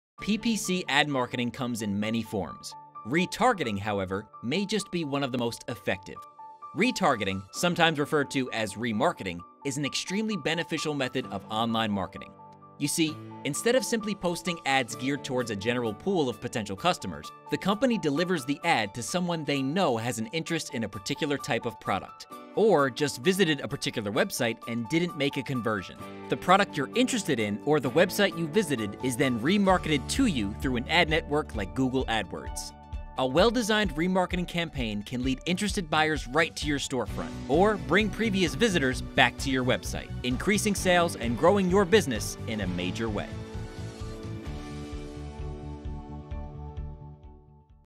What Is retargeting? | IBIS Studio SEO in Coral Gables.
“Retargeting” is literally re-targeting people; showing ads only to people who visit your website. They were interested in a product or service you have to offer–that’s why they visited your site in the first place. Your ads will appear on other publisher’s websites as a reminder to come back to your website.

It’s an easy, simple, friendly, inexpensive way to get more customers who don’t convert when they first check out your website. Call us to learn more about the varieties of Pay Per Click campaigns.